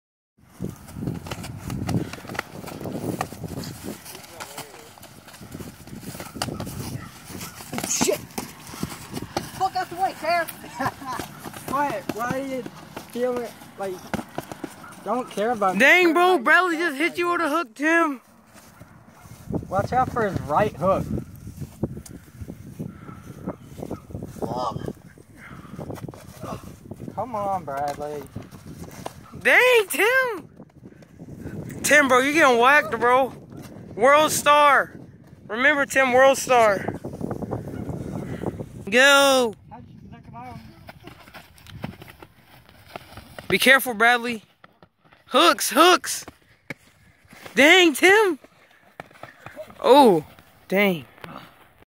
Revenge for hitting me in the snow lol worldstar boxing match😂💯💯👊
I wasnt done but he said he was so ig ww finished our beef😂😂😂nothing to serious